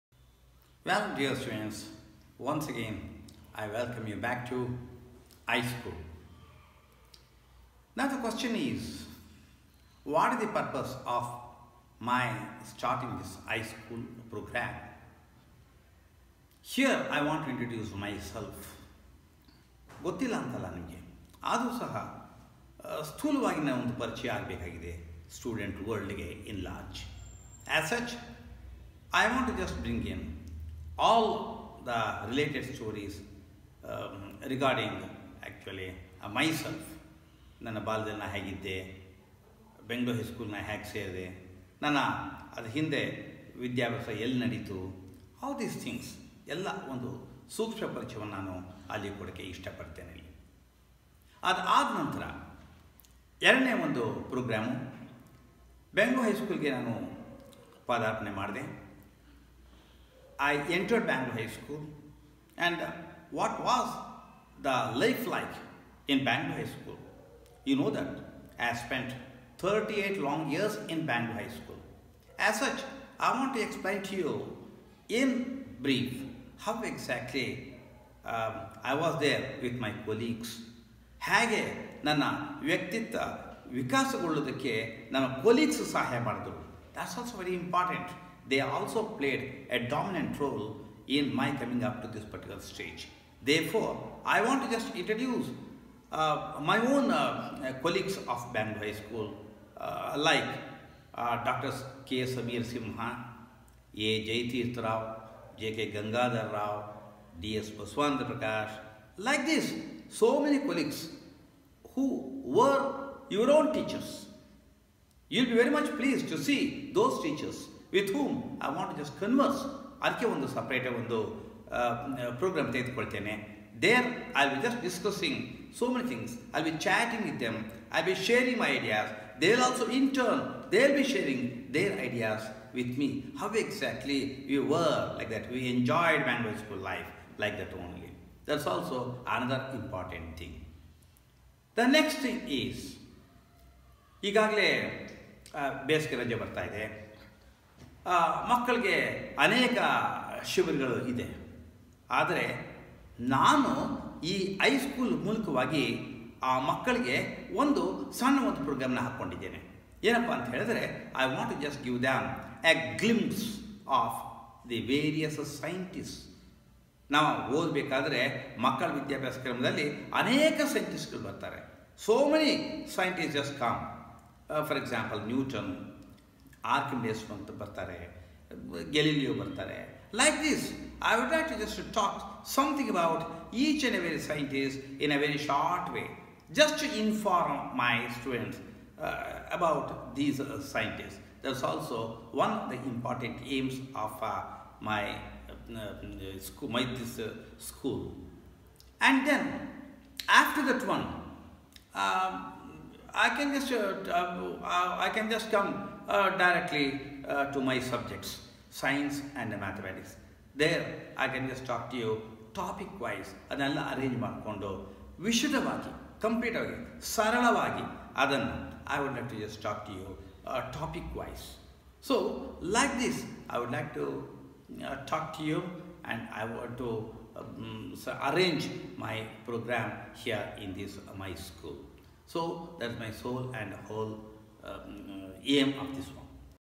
BVN iSchool Programs for 2018
BVN enumerates various programs envisioned on this channel. He talks about his idea of presenting some untold fond memories in his life journey. He also presents the thought of reliving his special moments with dear Colleagues of his. He wants to share interesting stories of various Mathematicians and Scientists. Lastly, he wants to bring the topics of choice to his students in Science and Maths to provide strong fundamentals to the community across the world.